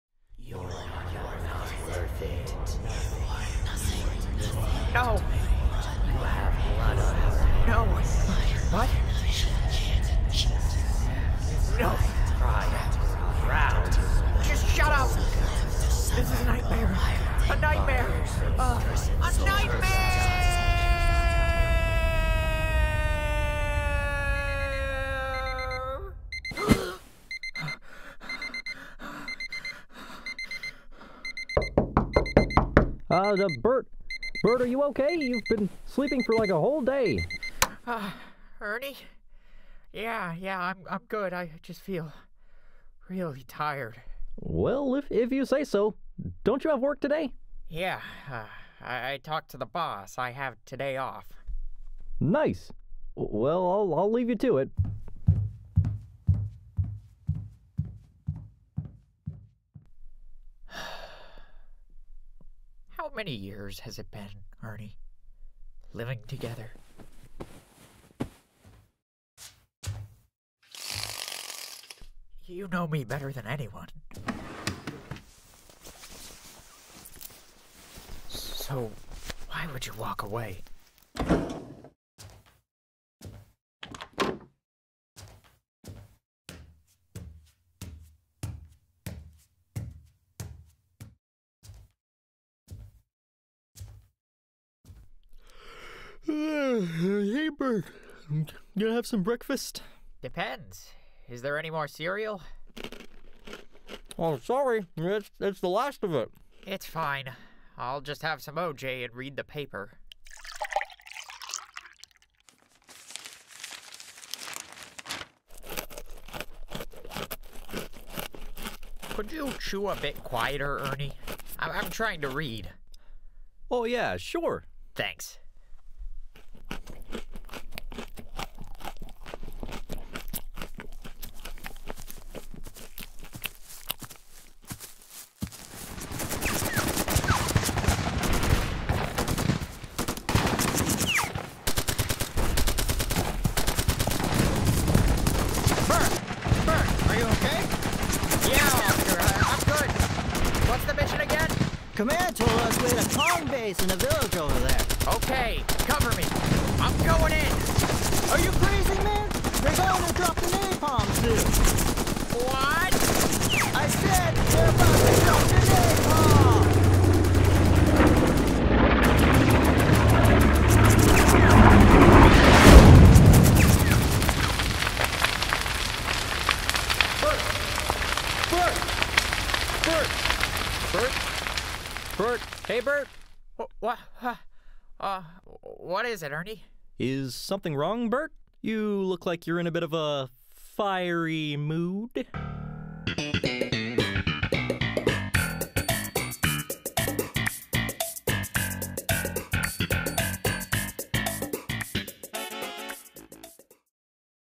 A r/Bertstrip Experience
An audio project that turned into a shitpost. Also an homage to r/bertstrips, maybe. Hope ya like it.